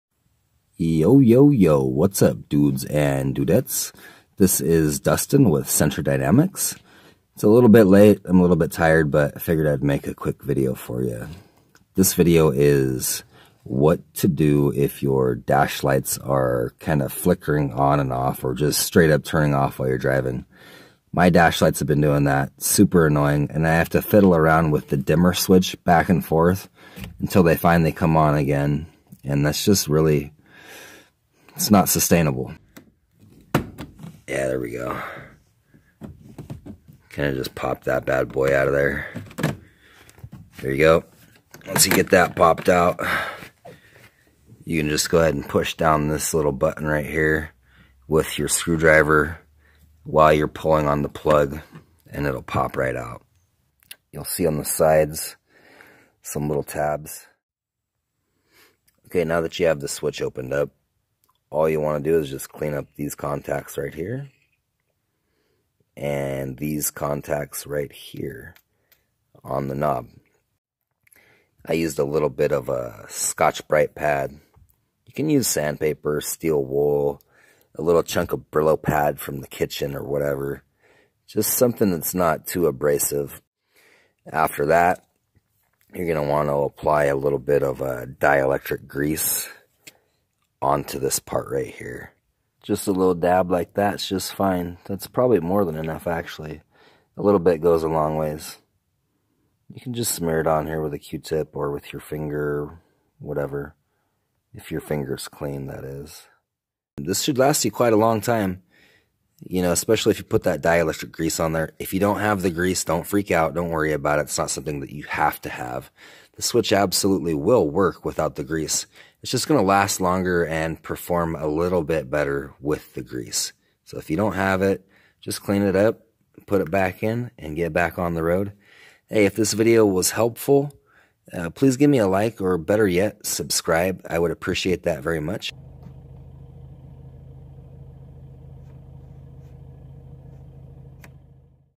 Flickering Dash Lights. Easy DIY fix
This applies directly to a 95-99 Nissan Sentra.  Most other dash light dimmer switches can be cleaned in a similar fashion. Take your time and chances are you can figure it out. You rock : )